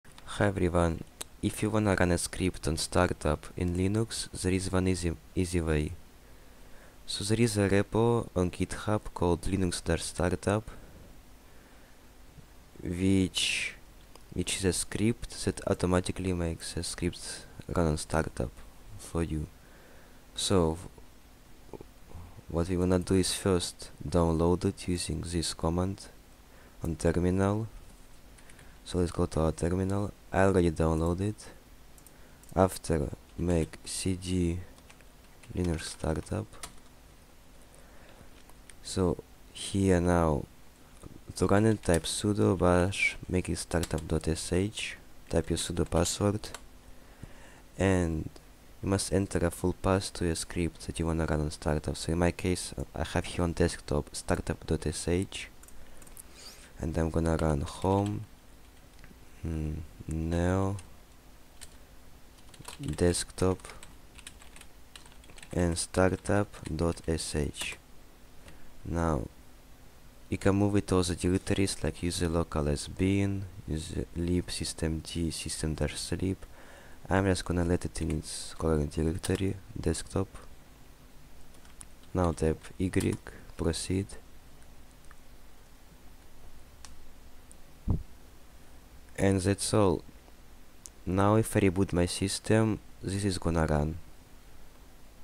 How to run a script on Startup using Systemd
In this video I'm showing how to run any interpreted  scripts (like Python, Bash, Ruby, etc...) on startup using Systemd in a pretty easier way.

Also don't forget that your startup script has to have a Shebang on the first line.

If your init system isn't Systemd, it won't work.